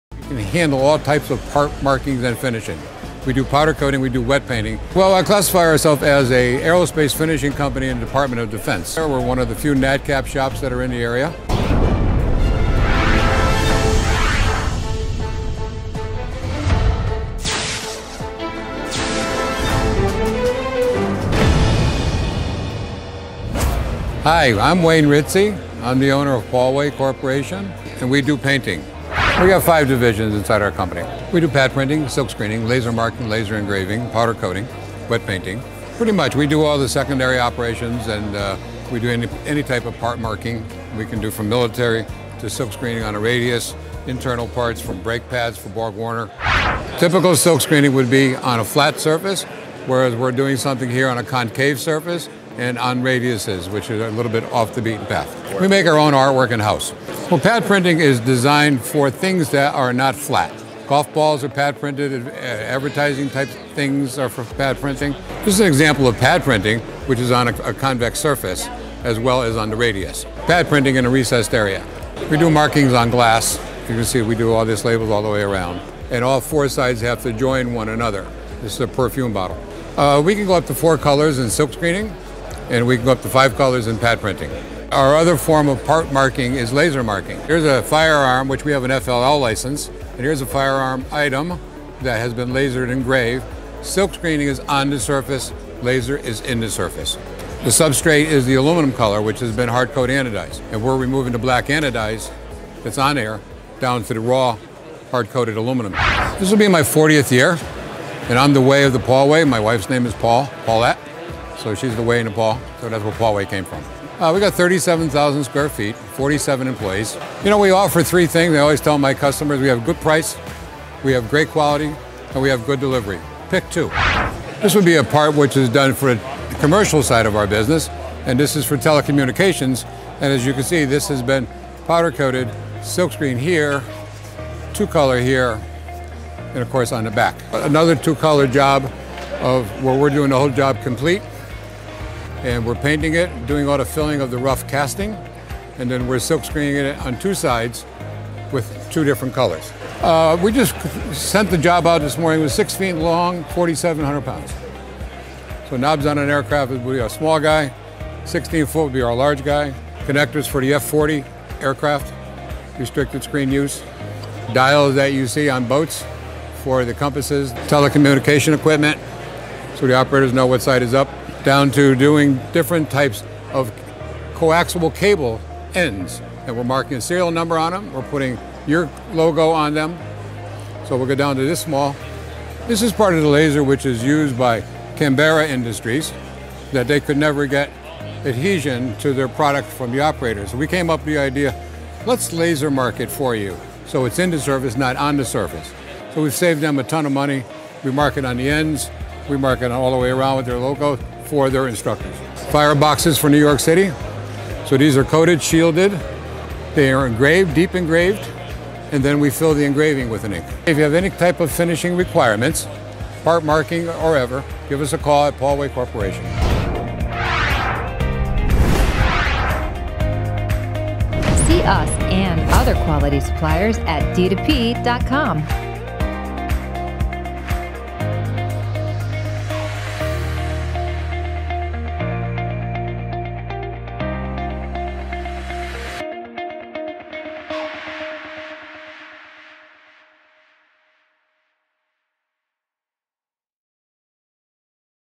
Industrial Painting & Mil-Spec Coatings | Pauway Corp | Wallingford, CT
Need mil-spec painting or coating? How about silk screening or pad printing? Pauway is the company to watch. Pauway is a featured supplier at the Design-2-Part Shows

Since 1979, Pauway Corporation has earned a reputation as a quality provider of industrial painting, powder coating and screen printing services to diverse industries including Aerospace & Defense, Firearms,  Electronic, Medical Device, and OEM  throughout New England and nationwide.

Superior Preparation - Unmatched Craftsmanship

        Industrial Painting
        Silk Screening
        Wood Finishing
        Pad Printing

    Aerospace & Mil-Spec Coating
    Powder Coating

Pauway proudly now offering Cerakote coatings!